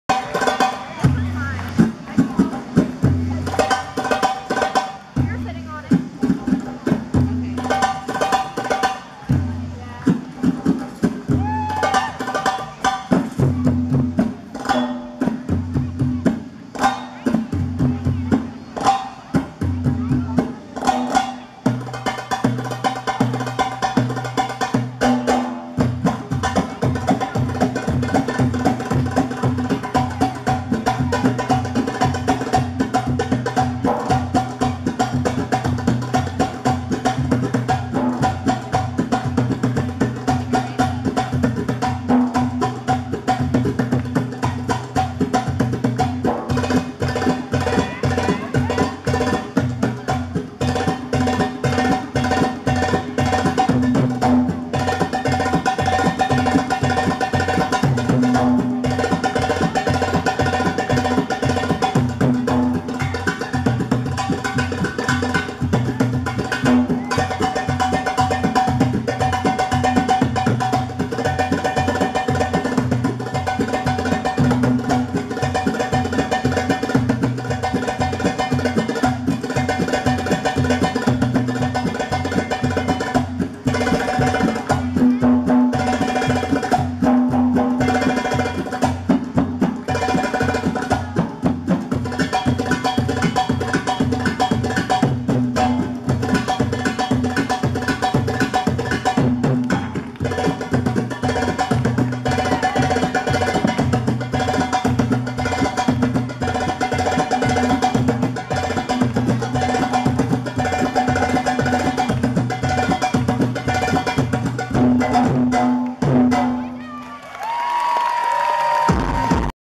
Drum Choreography....Shimmy
An intricate drum choreography performed by an amazing group of eight performers from
Aalim Dance Co. aka Aalim Belly Dance.